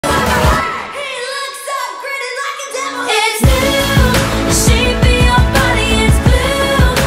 So cute!
I love a good football/celebrity romance. Their story reminds me so much of Danny and Jennifer in my books.